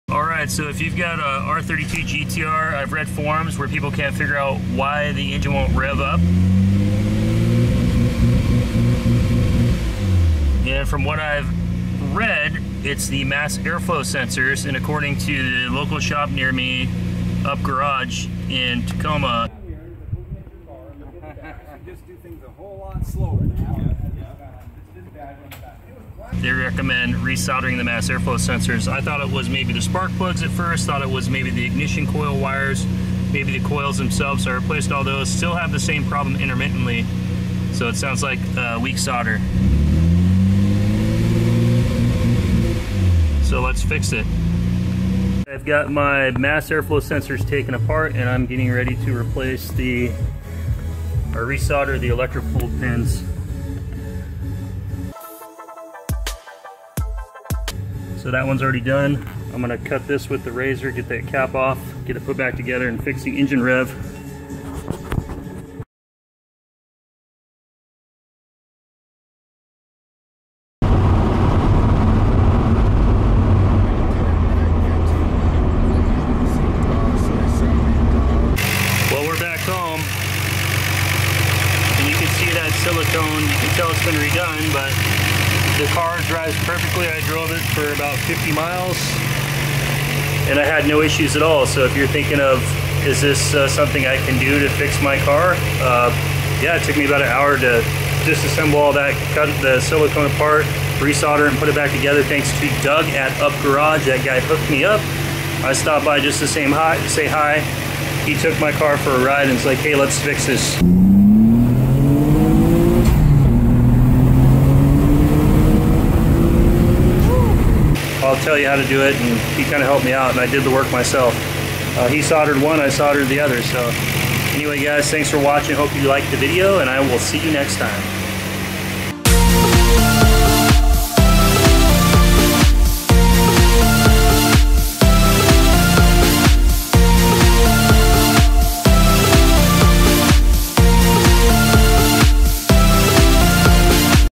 RB26DETT engine wont rev nissan skyline ignition problems bad maf sensor repair
Nissan Skyline R32 R33 R34 engine wont rev above 2500 RPM. How to fix your RB25DET RB26DETT engine when it wont rev above 2500 RPMs. Nissan Skyline stock MAF's are old and many other Nissan Engines suffer from aging mass air flow meters. RB25det is known for same issue. Fixing your ignition problems on your Skyline R32 R33 R34 rb20 rb25 rb26 is an easy task. All you need is some RTV and a razor blade to make the repair.
Just remove RTV from around both Air flow meters and remove the plastic cap covering the sensors brain.  The solder becomes weakened over time from being heated up and cooled down every time you run the car and turn it off.  The ecu is not reading the sensors so it goes into safe mode not allowing the engine to rev above 2500 rpm. If you have been driving down the freeway and all the sudden you car starts jerking really hard. This is just a weak MAF solder. Look inside the MAF, you will see 4 small pins inside that will need to be re soldered to reestablish contact to the chip. Repairing your mass air flow MAF sensor is a very easy fix. Only takes about an hour. I thought there was something really wrong with my car. I thought maybe my Rb26dett engine was blown. I had this issue intermittently for the first year I had my car. It made the car almost no fun to drive because I never knew if it was going to run right. Thanks to Doug at UP Garage in Tacoma WA, he educated me on the problem helped my fix this issue on my car. UP Garage hooked me up and showed me some kindness, thanks Doug!

My Gear
Main Camera Sony
Backup Camera Pixel 3A
Camera handle grip
In car camera mount